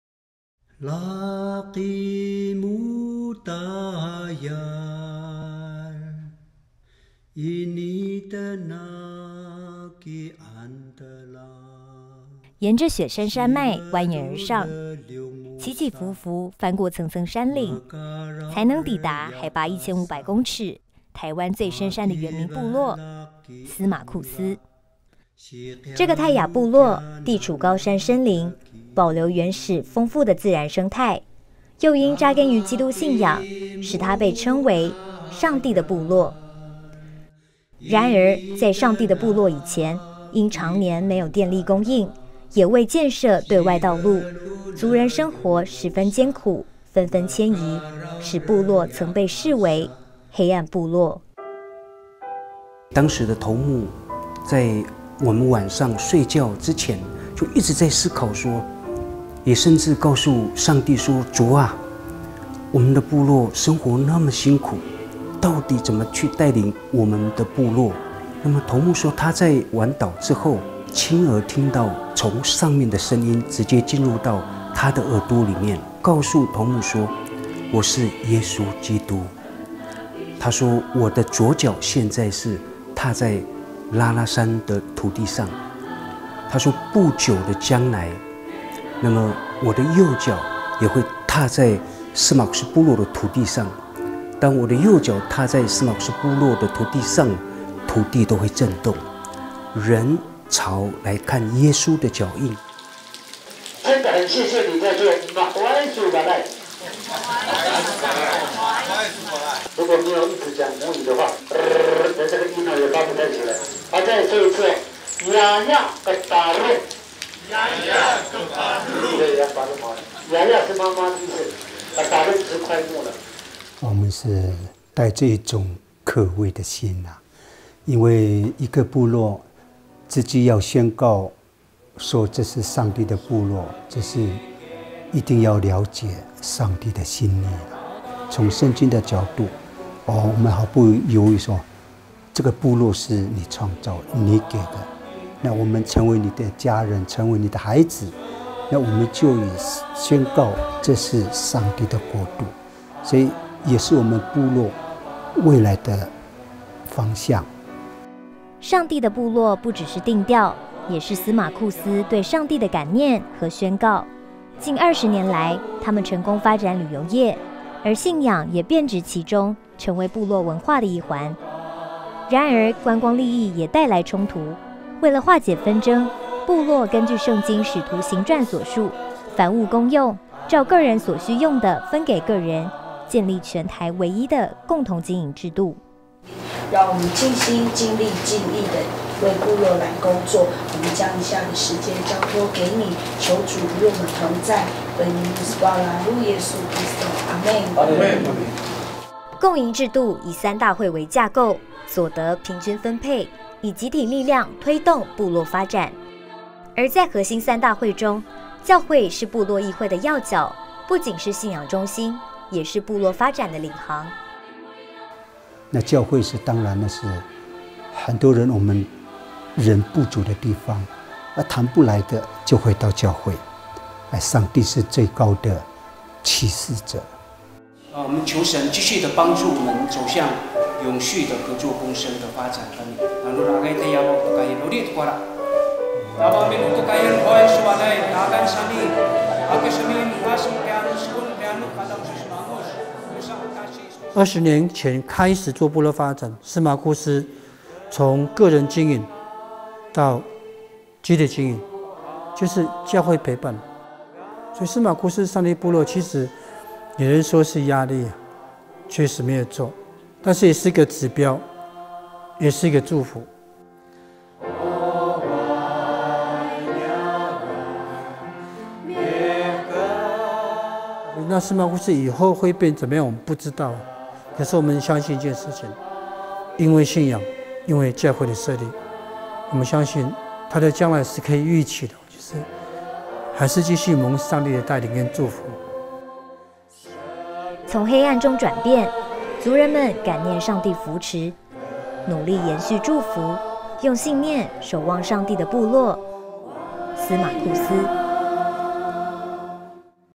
【好消息國度報導】自立興起 司馬庫斯共營成就上帝部落
沿著雪山山脈蜿蜒而上，起起伏伏翻過層層山嶺，才能抵達海拔1500公尺，台灣最深山的原民部落，司馬庫斯。

這個泰雅部落地處高山深林，保留原始豐富的自然生態，又因扎根於基督信仰，使它被稱為「上帝的部落」。然而，在發展觀光以前，因長年沒有電力供應，也未建設對外道路，族人生活十分艱苦，紛紛遷移，使部落曾被視為「黑暗部落」。
「上帝的部落」不只是定調，也是司馬庫斯對上帝的感念與宣告。近20年來，他們成功發展旅遊業，而信仰也遍植其中，成為部落文化的一環。然而觀光利益也帶來衝突。為了化解紛爭，部落根據聖經《使徒行傳》所述「凡物公用、照各人所需用的分給各人」，建立全台唯一的共同經營制度。共營制度以三大會為架構，所得平均分配，以集體力量推動部落發展。而在核心三大會中，教會是部落議會的要角，不僅是信仰中心，也是部落發展的領航。
從黑暗中轉變，族人們感念上帝扶持，努力延續祝福，用信念守望上帝的部落─司馬庫斯。